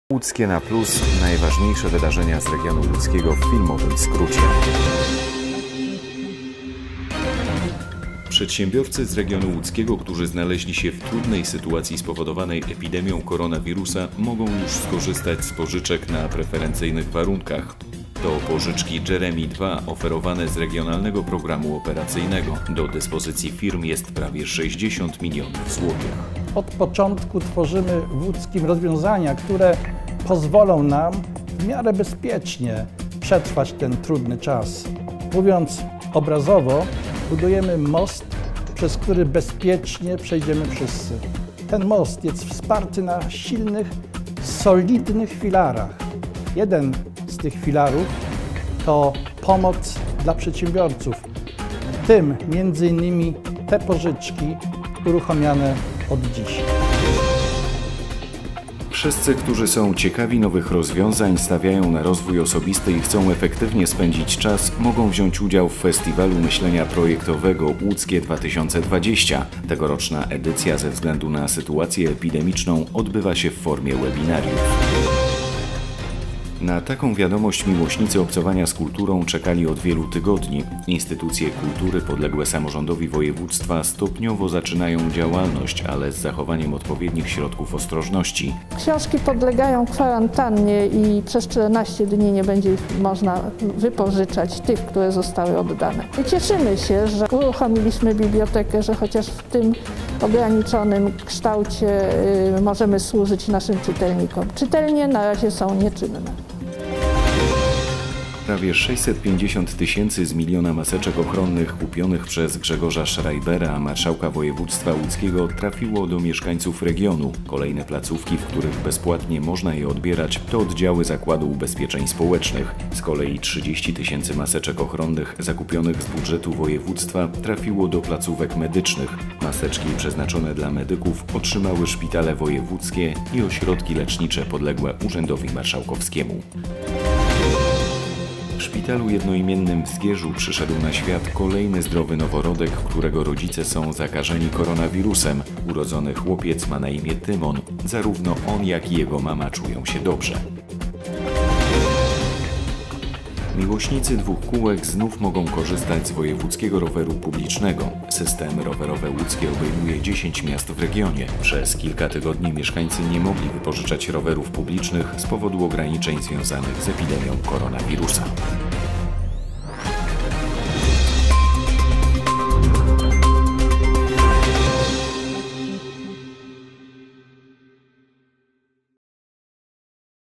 Łódzkie na plus #15
W 15. odcinku o:
💰 Pożyczkach Jeremie 2 dla przedsiębiorców
✅ Festiwalu Myślenia Projektowego
❤️️ Instytucjach kultury  - znów otwartych
(Pasjonaci czytelnictwa mogą już odwiedzać w Łodzi: Bibliotekę Wojewódzką im. Marszałka Józefa Piłsudskiego oraz Pedagogiczną Bibliotekę Wojewódzką im. prof. Tadeusza Kotarbińskiego.
Otwarte są także: łódzkie Muzeum Sztuki i Muzeum Pałac Herbsta. Z kolei 20-ego maja planowane jest otwarcie Muzeum Archeologicznego i Etnograficznego w Łodzi i Skansenu „Łęczycka Zagroda Chłopska”.)
✅ Kolejnych partiach rozdanych maseczek
🍼 Narodzinach zdrowego noworodka (zakażonych rodziców) !
🚴 Rowerowym Łódzkim - znów działa